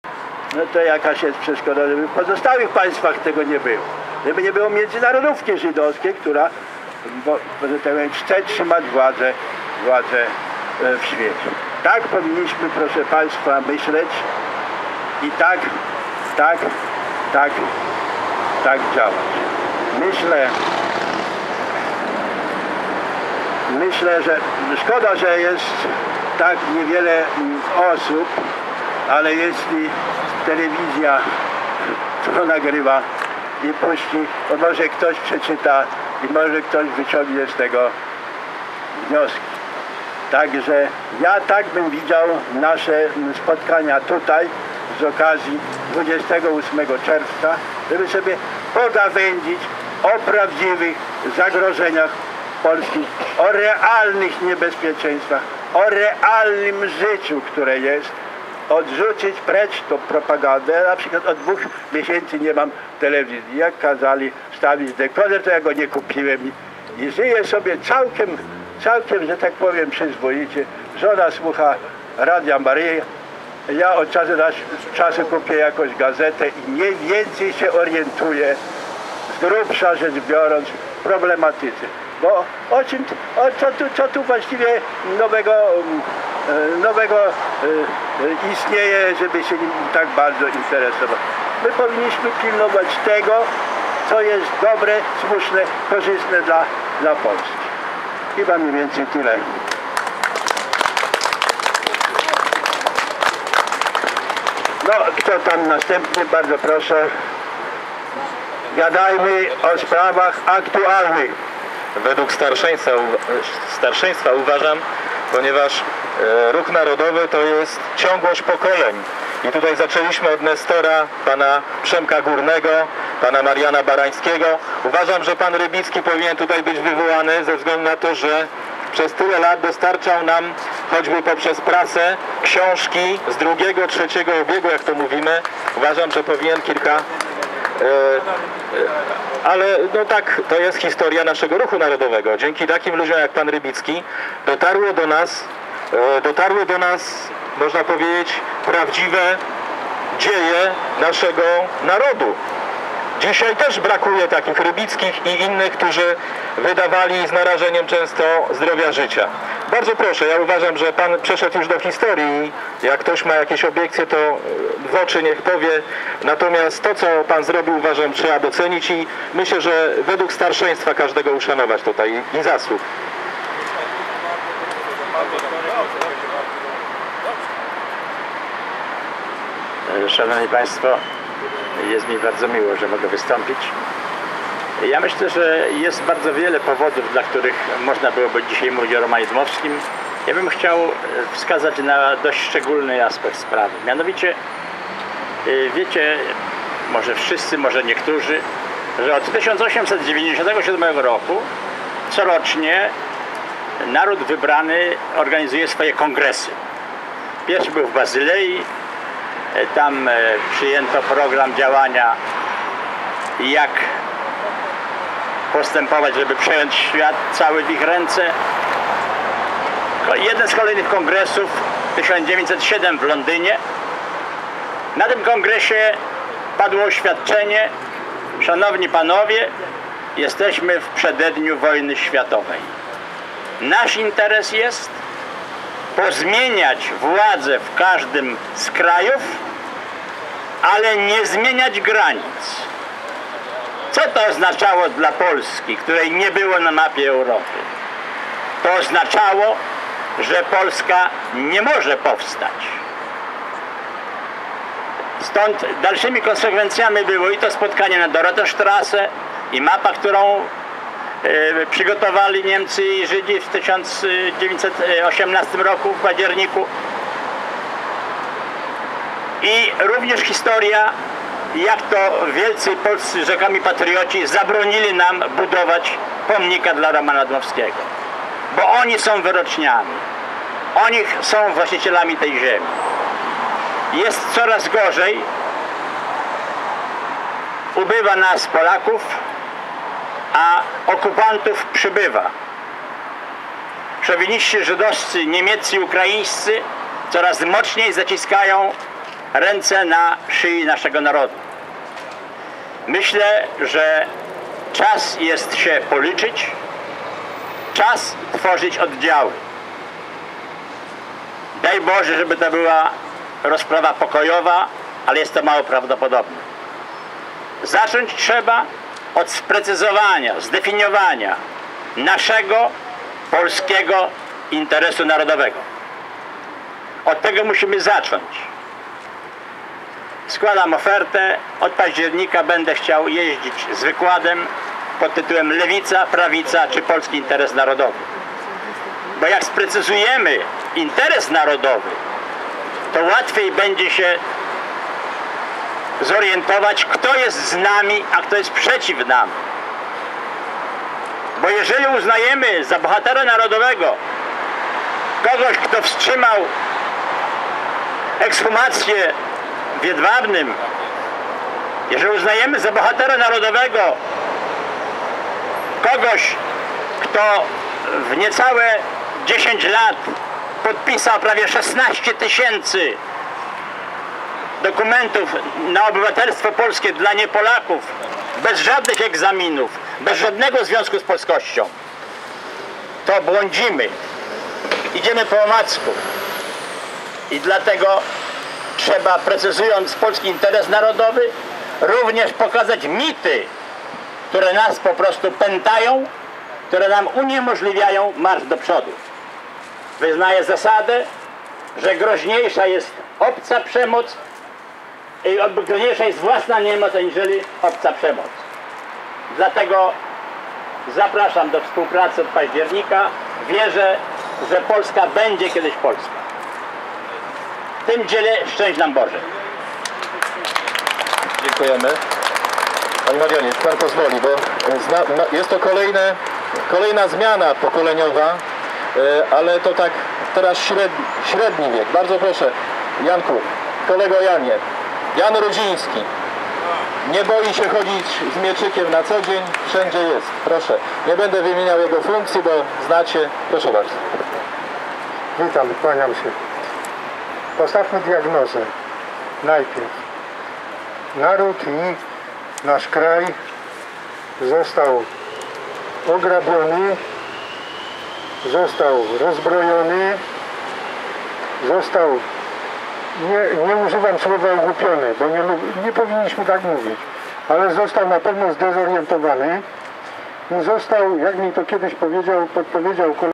28-06-2013 BOGUSŁAW RYBICKI PRZEMAWIA A MARIAN BARAŃSKI PROTESTUJE POMNIK DMOWSKIEGO WARSZAWA
28 czerwca 2013 godz.18:05  - rocznica Traktatu Wersalskiego przy pomniku Romana Dmowskiego w Warszawie (Plac Na Rozdrożu). Sławomir Zakrzewski prowadzący program uroczystości udziela głosu Bogusławowi Rybickiemu, prezesowi Stronnictwa Narodowego "Ojczyzna", co spotyka się z nerwową reakcją Mariana Barańskiego, prezesa Stronnictwa Narodowego "Szczerbiec". Bogusław Rybicki był nauczycielem ideowym młodego Wojciecha Wierzejskiego, kiedy w latach 90-tych kandydował z białopodlaskiego do parlamentu. Człowiek historia. Ten moment gniewnego zachowania Prezesa Barańskiego to też jest historyczny gest, wspomnienie i echo tamtych zażartych gniewnych sporów i wojen miedzy nimi w latach 90-tych XX-go wieku. Ich historyczny spór konkurencyjny zakończony został przez wejście na rynek idei i organizacji narodowej Ojca Tadeusza Rydzyka i Romana Giertycha, którzy ich "pogodzili" odbierając im elektorat, wpływy i znaczenie wśród szerokich mas...
Bogusław Rybicki zasłynął pomysłem, żeby połączyć funkcję prezydenta i premiera. Wydawał prasę narodową, uświadomił politycznie wielu Polaków.